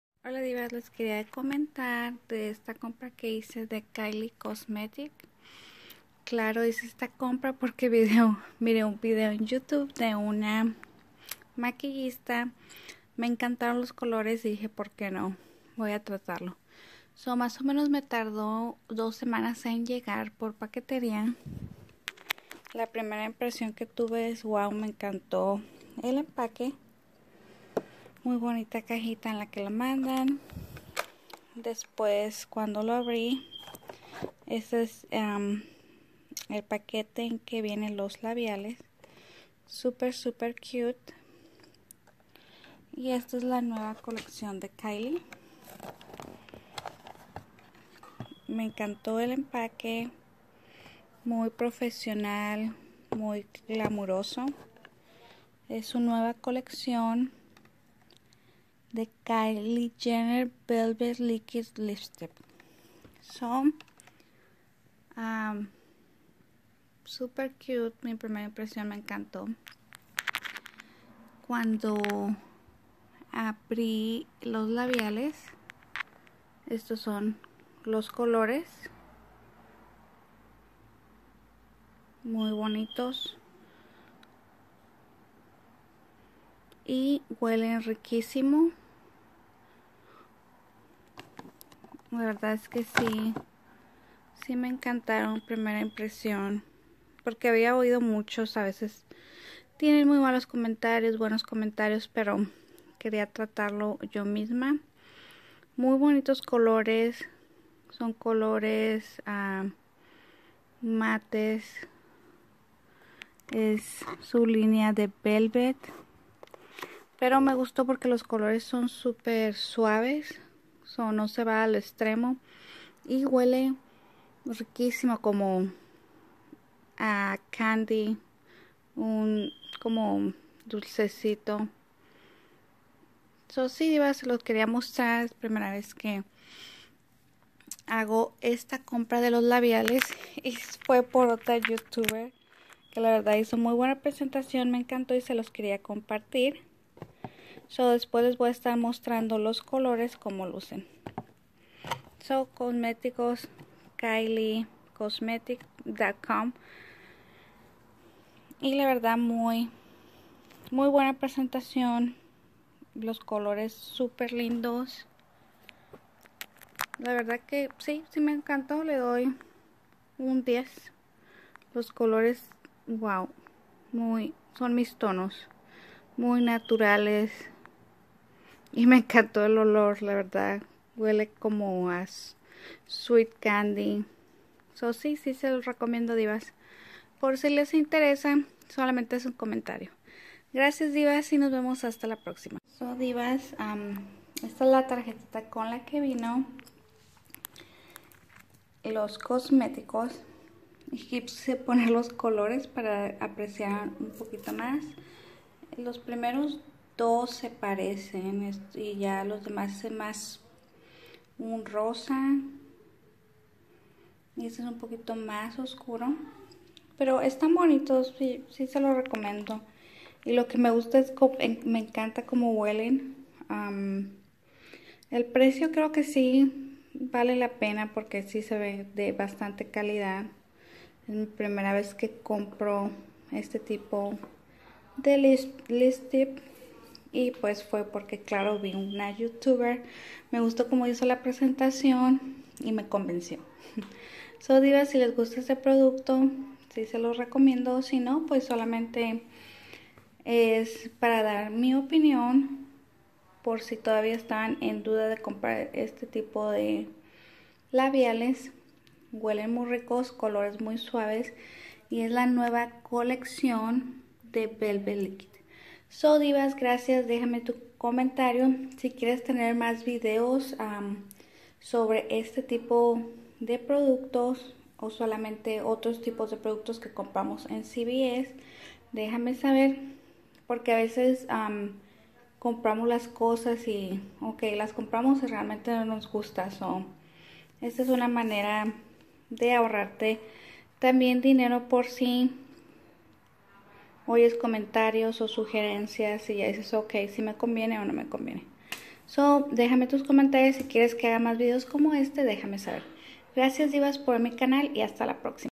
DIVAS KYLIE COSMETICOS ME GUSTO O NO💋😗
DIVAS MI OPINION EN LOS LABIALES DE KYLIE MUY POPULARES💋